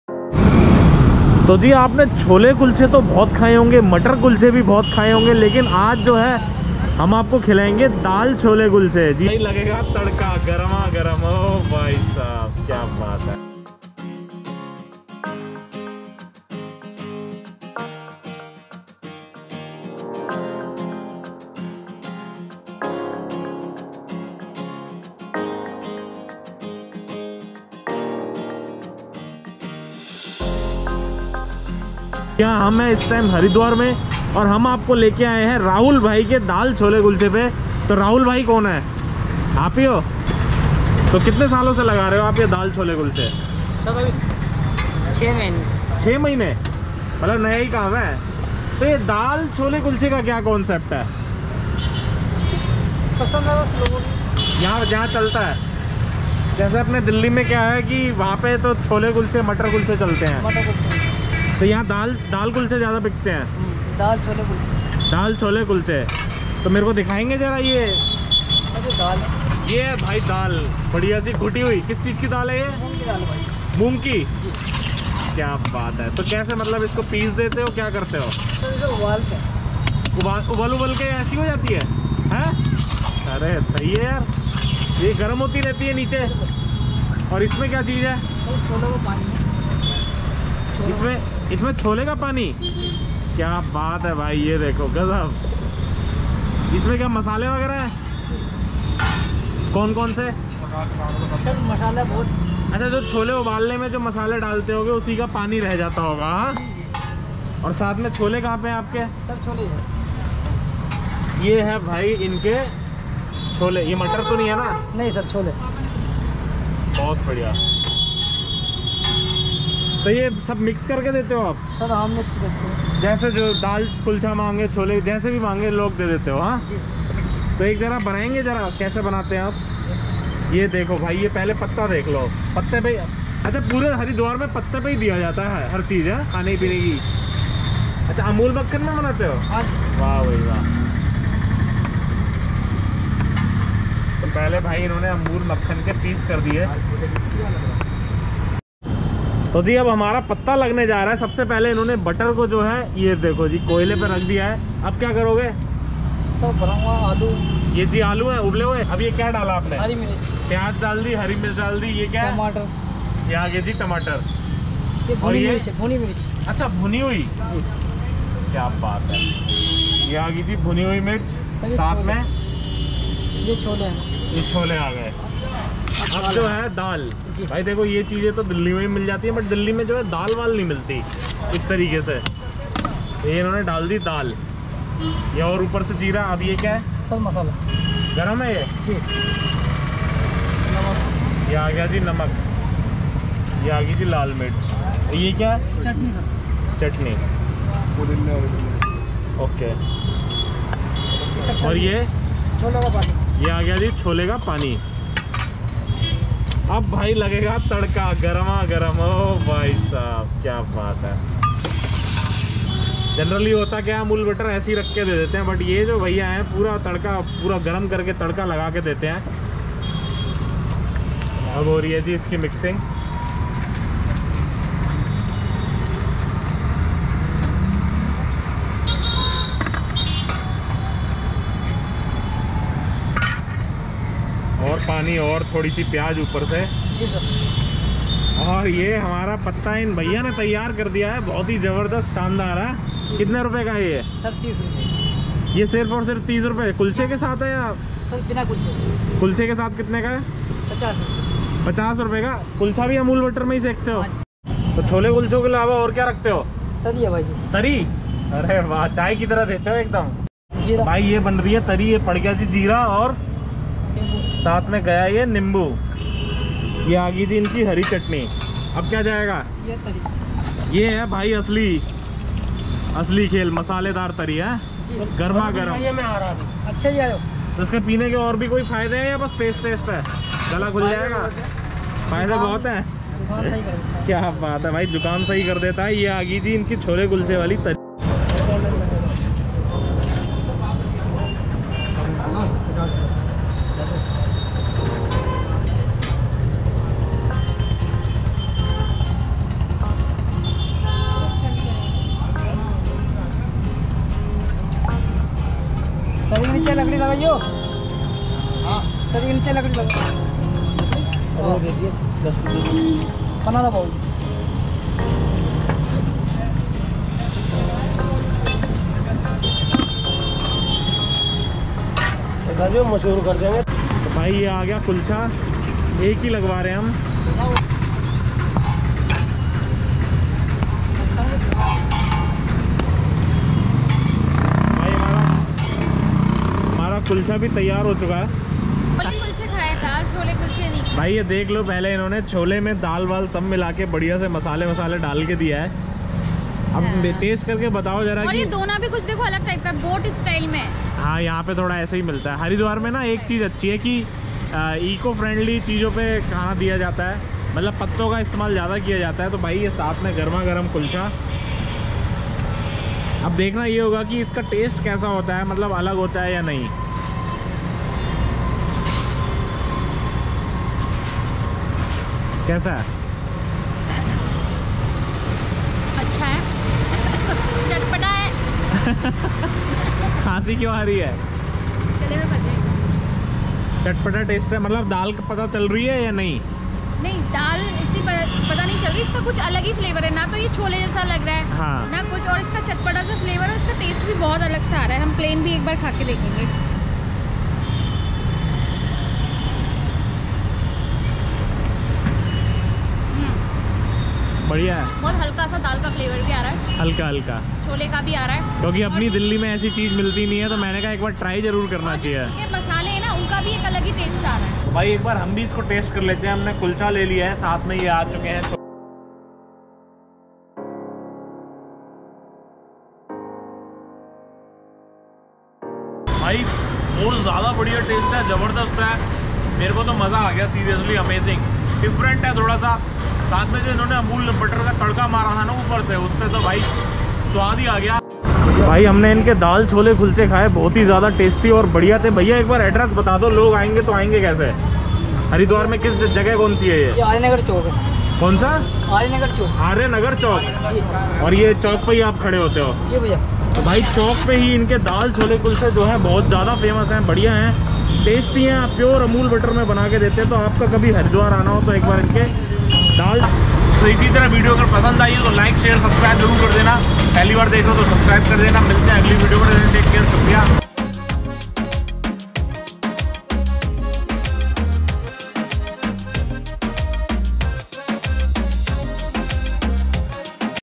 Haridwar के मशहूर Unique DAL छोले कुलचे और soup । Street food india
this guys sells unique chole kulche dal chole kulche in Haridwar street food of india